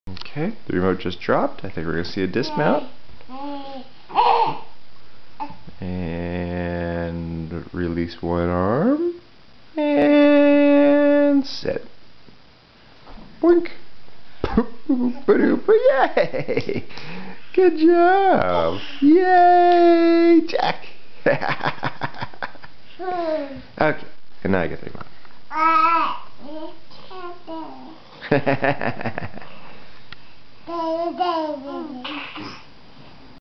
Frank does a play by play as Jack sits
Jack perfects his dismount as Frank narrates...very cute!